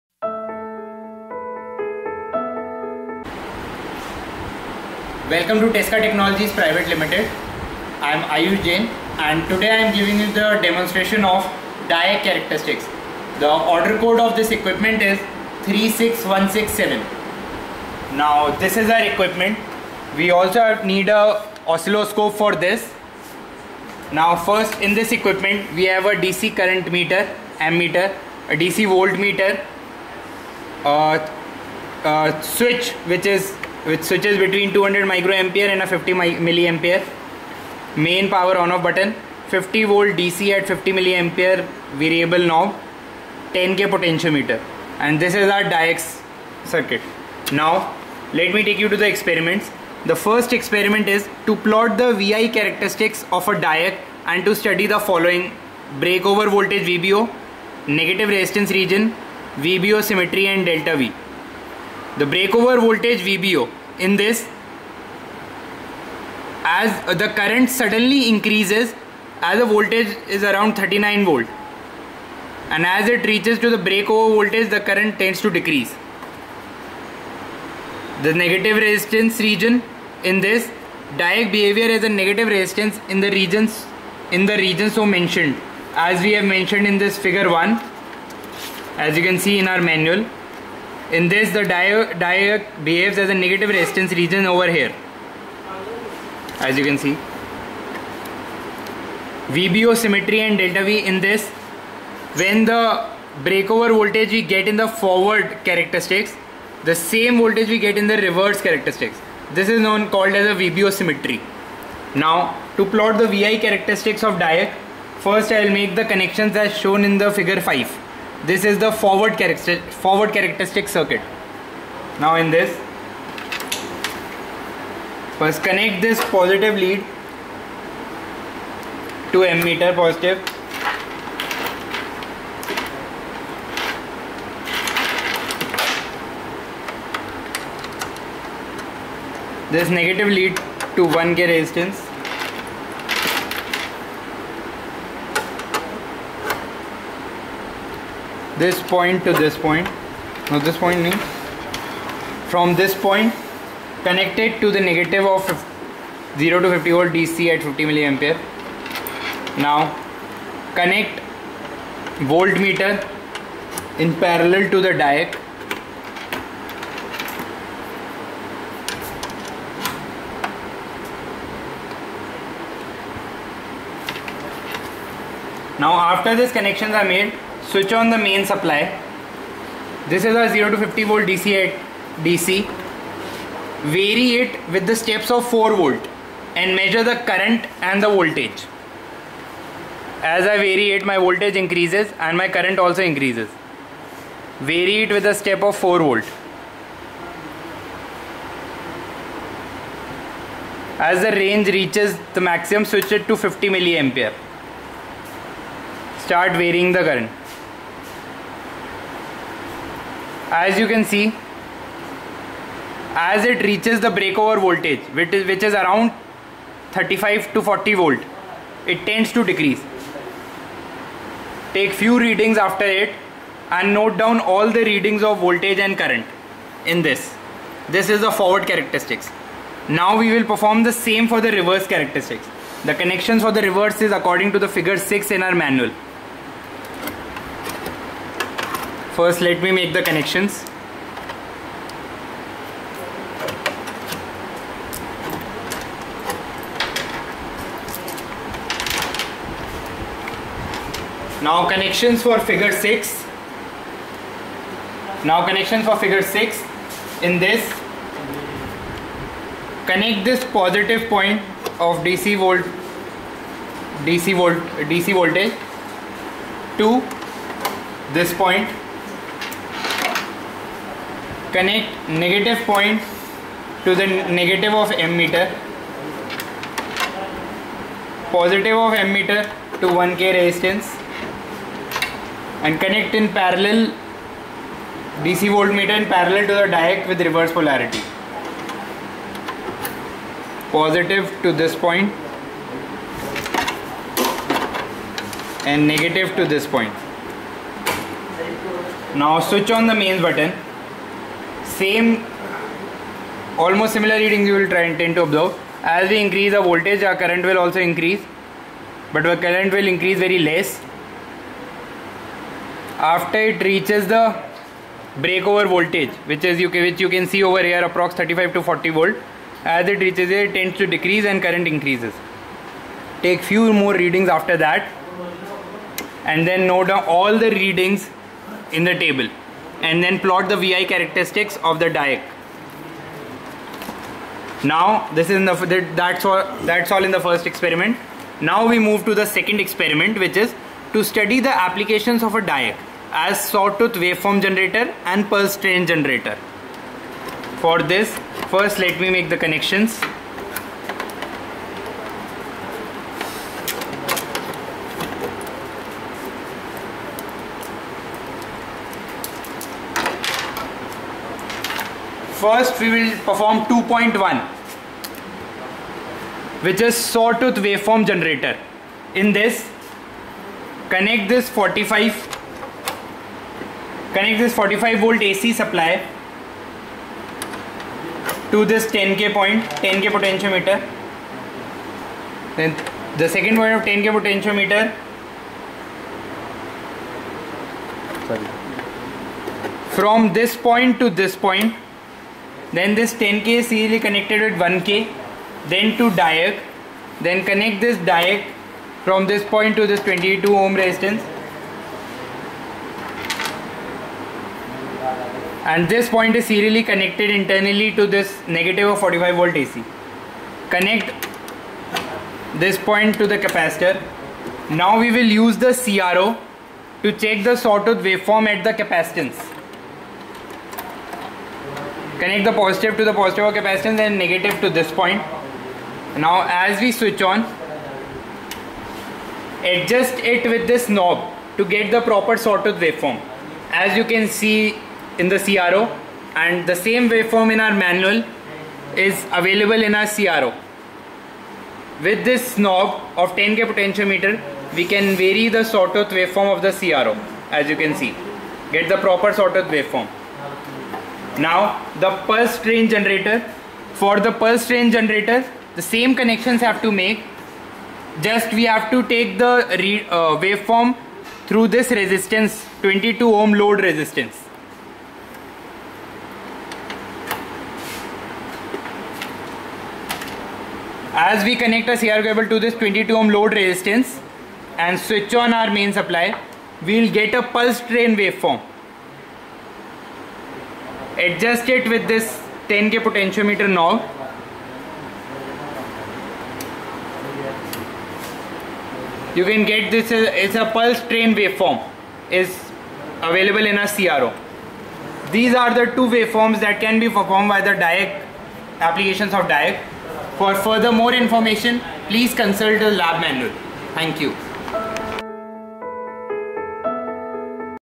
Diac Characteristics with power supply and 2 meters | Power Electronics || Tesca 36167
In this video, we described Diac Characteristics with power supply and 2 meters.